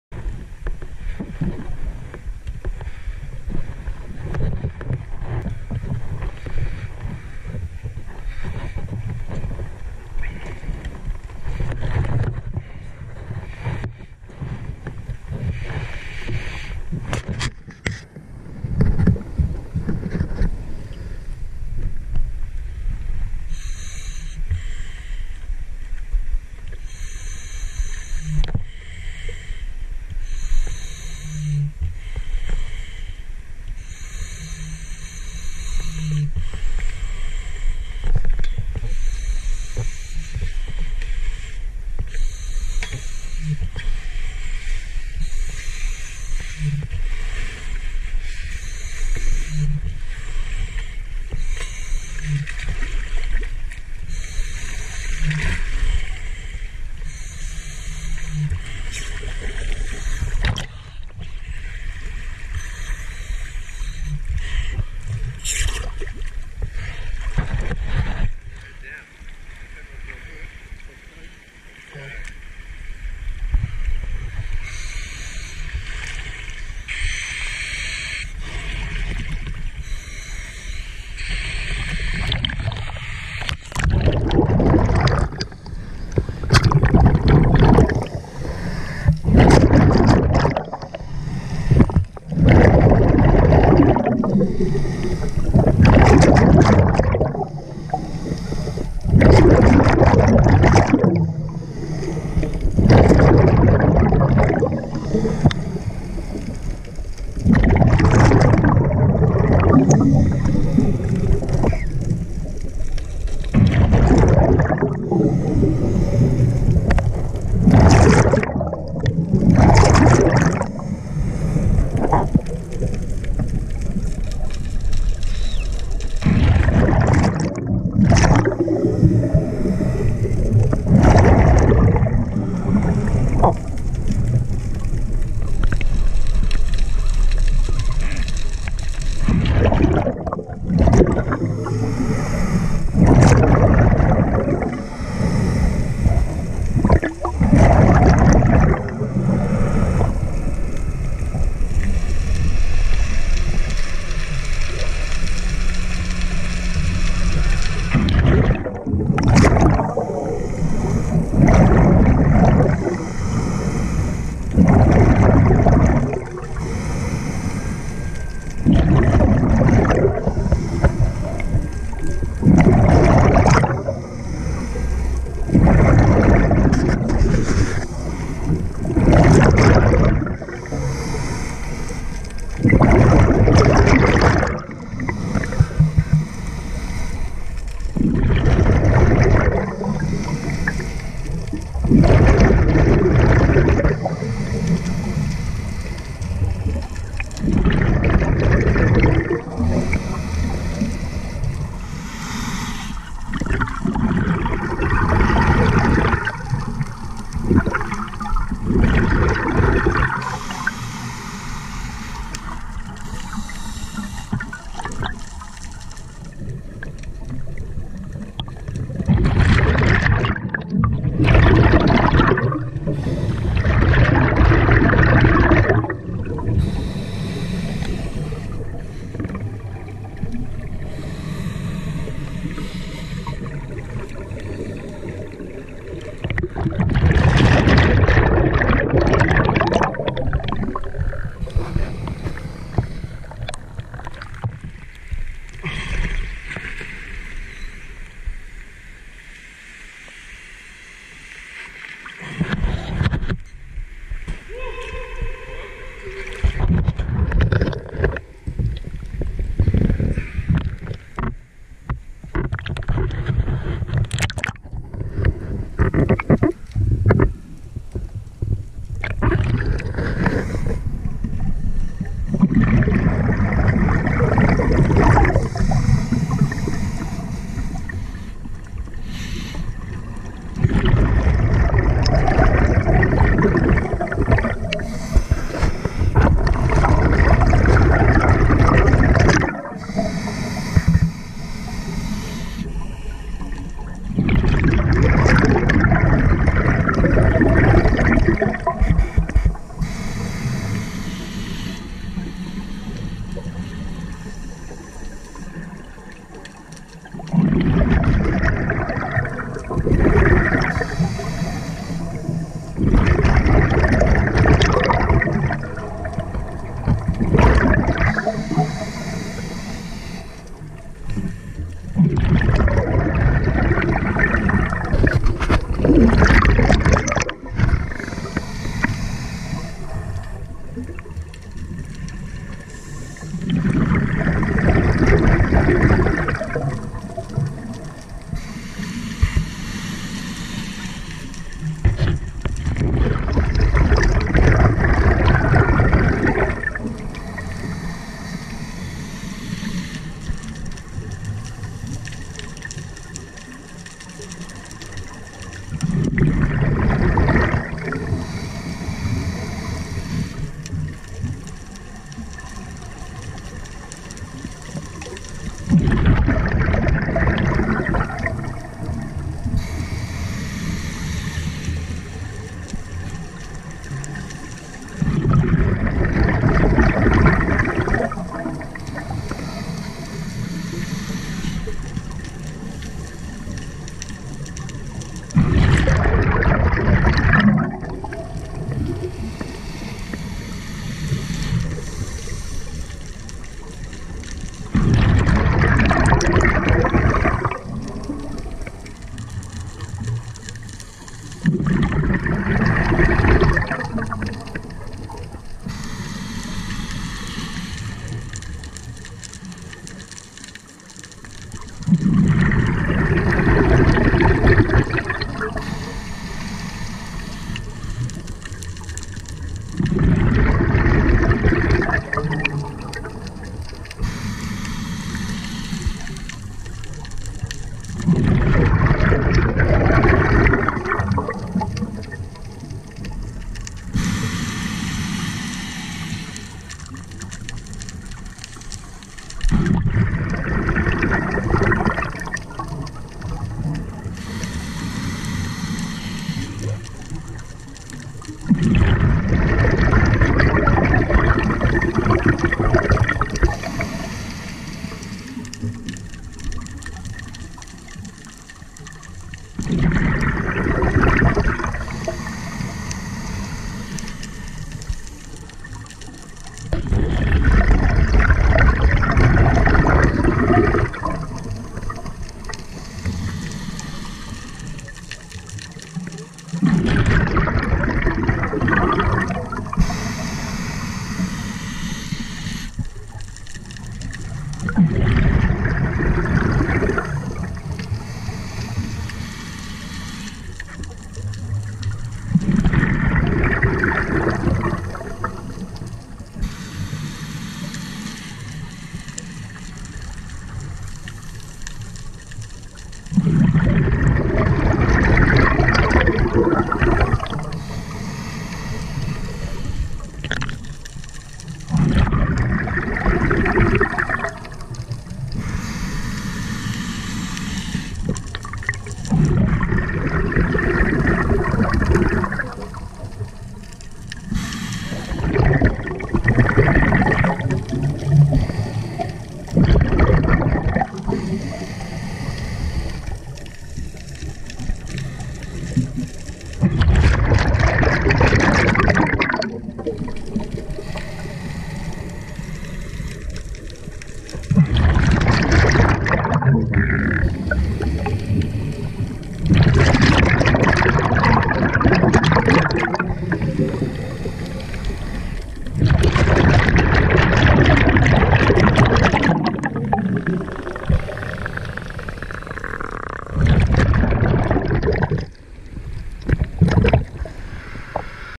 Cathedral Cave (Ghar ir Rih) Gozo Scuba Dive
A dive to Cathedral Cave in Gozo in July 2019 - Total dive time 40 minutes with the deepest part at around 17 meters. This video does not do the cave justice due to poor light in the cave. This is a must see if you get the chance.